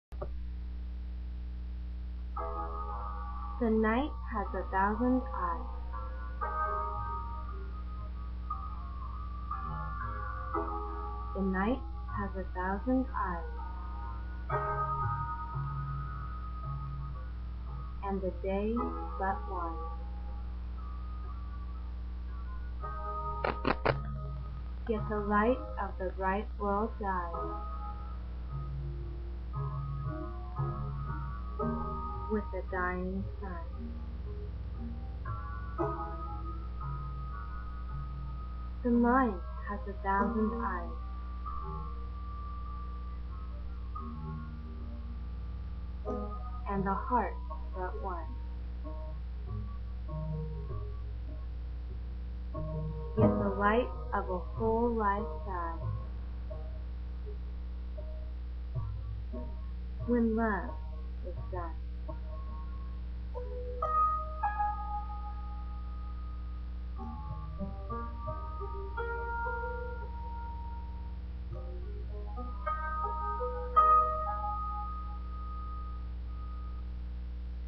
The Night has 1000 Eyes Narrated
Narrated movie of the Poem, "The Night has a Thousand Eyes", by Francis William Bourdillon. This movie is a project for my Educational Technology class at San Diego State University.